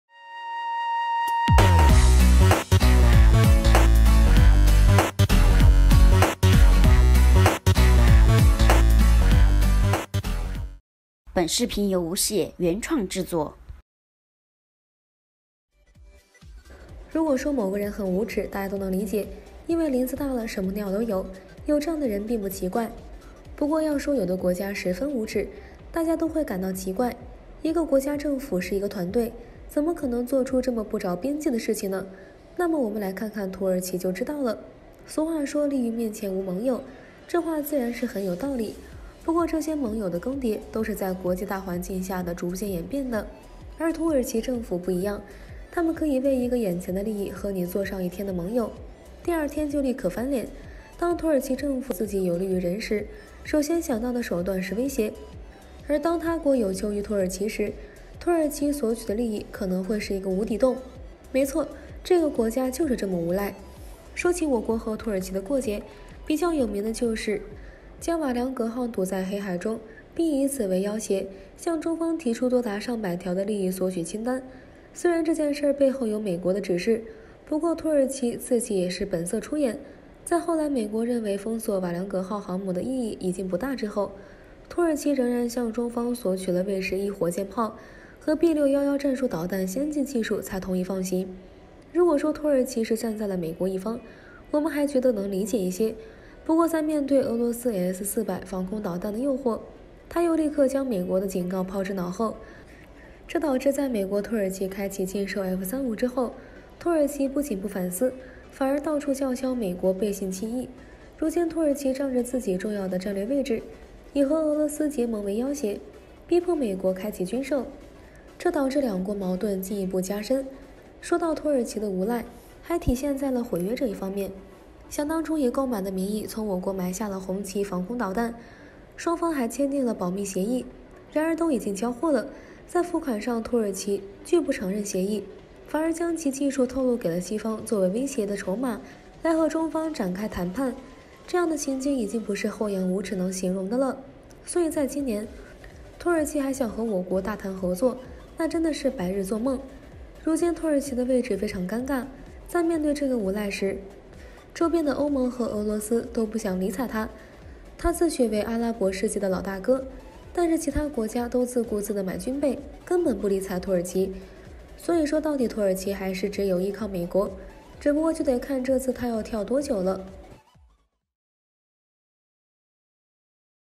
土耳其請求和中國合作，網友：先把舊賬算一下
如果說某個人很無恥，大家都能理解，因為林子大了什麼鳥都有，有這樣的人並不奇怪，不過要說有的國家十分無恥，大家都會感到奇怪，一個國家政府是一個團隊，怎麼可能做出這麼不著邊際的事呢？那麼我們看看土耳其就知道了。